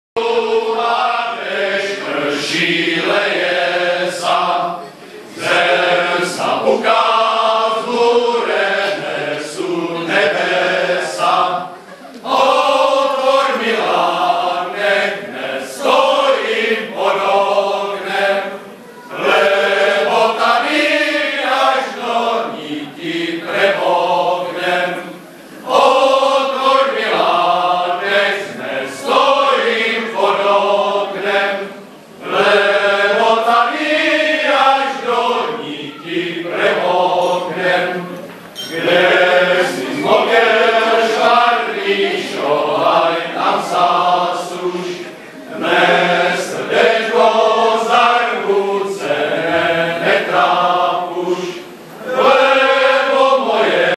Mužáci z Tasovic u Znojma
Historicky první vystoupení mužáků z Tasovic na Vinařském Tufru 2012.